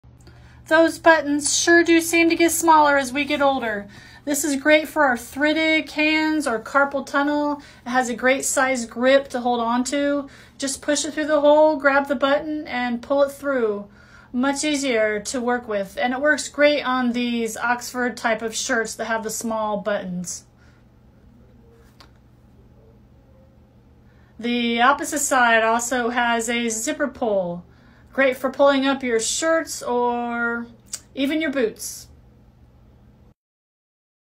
Button tool and zipper assist
Hands having a hard time buttoning those little buttons?  This will help, especially those small shirt buttons.  Also has a hook to use as a zipper pull in the other end.